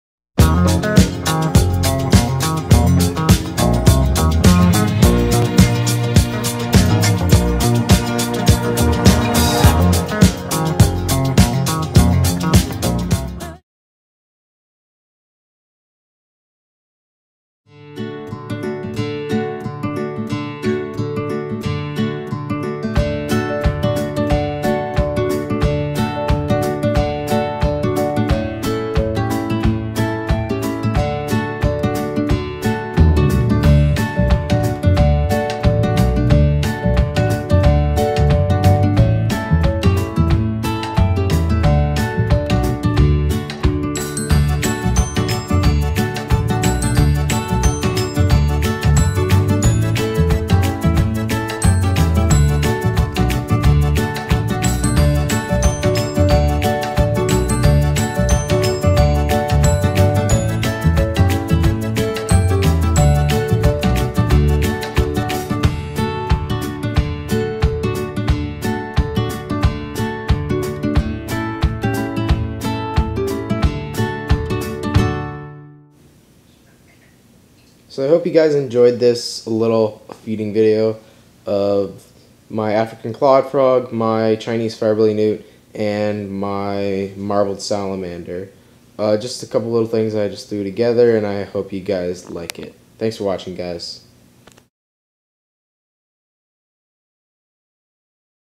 Christmas Dinner Feast
My first ever Christmas video for this channel. Hope you guys enjoy this quick feeding video.